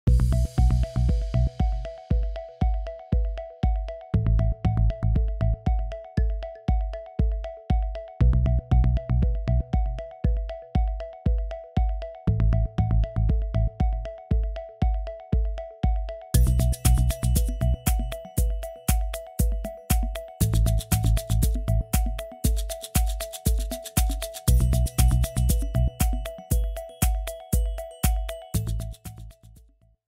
LED Baseball cap
This fantastic baseball cap has a LED strip all around the front edge. The strip has two different light modes, flashing or fixed. It's bright and very visible, perfect to catch the attention everywhere!